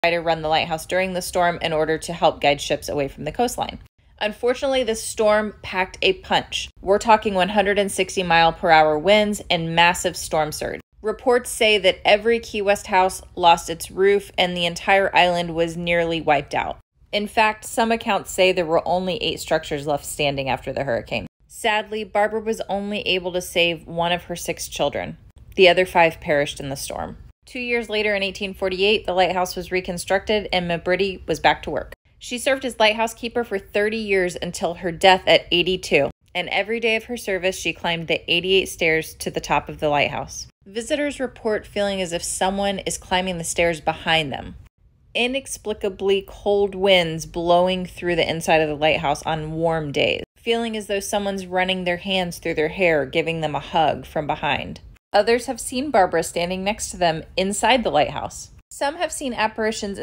Number 8 Most HAUNTED Place in Key West: THE LIGHTHOUSE
PART 2: Barbara Mabrity was a strong lady who was endlessly committed to her job as Key West’s lighthouse keeper. (So commited she’s still climbing those steps to light the lantern 200 years later!) After inheriting the job from her husband upon his tragic death to yellow fever, Barbara picked up the baton and took off running. She prided herself on keeping mariners safe. In 1846 a category 5 hurricane struck Key West. It was called the Havana Hurricane and it packed winds of 160+ mph along with absolutely devestating storm surge. The lighthouse sheltered Mabrity, her six children, and approximately seven other souls. When a large wave took out the lighthouse Mabrity was only able to save one of her children. The others perished along with the other souls sheltering in the lighthouse. Mabrity’s survival was understandably the stuff of lore. She was tough as nails. The island was nearly decimated but two years later in 1848 the new Key West lighthouse was constructed and Mabrity was back at it. She worked as lighthouse keeper for 30 years until her death at age 82. And for each of those years she climbed the 88 steps straight up to carry out her life-saving duties. Visitors have reported feeling someone following closely behind them up the stairs but when they turn around no one is there. Others have said they’ve heard giggles and felt someone run their hands through their hair. Some have seen Barbara standing next to them inside the lighthouse. Visitors have also seen two little girls, thought to be two of Barbara’s daughters who perished in the hurricane, happily playing on lighthouse grounds. More ominously some visitors have seen an angry man dressed in Civil War era military attire standing at the door to the lighthouse staring at them angrily. The lighthouse was a beacon of hope and safety with Barbara at the helm but the grounds and people who ran the lighthouse experienced significant tragedy. It’s no wonder so many spirits still inhabit the space today!